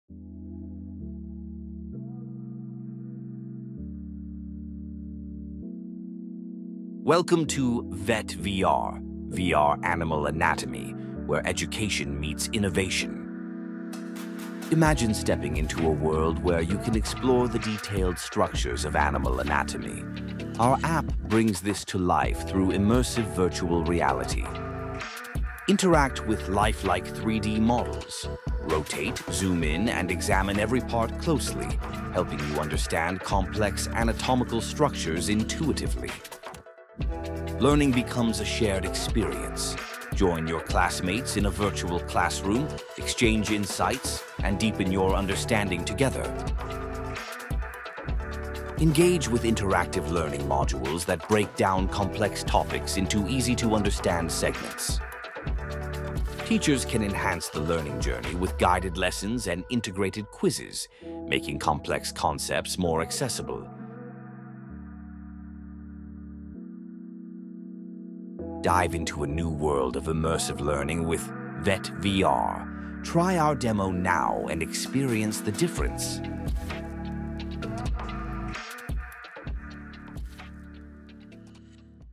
VET VR: Experience Veterinary Education Like Never Before
Traditional methods of studying animal anatomy can be overwhelming, but VET VR is here to help. Our VR app transforms complicated textbook content into a dynamic, interactive learning experience.

With lifelike 3D models and easy-to-navigate modules, you can explore animal anatomy like never before, making difficult concepts more accessible.

Don't miss out—try our demo today and discover how VET VR can revolutionize your learning journey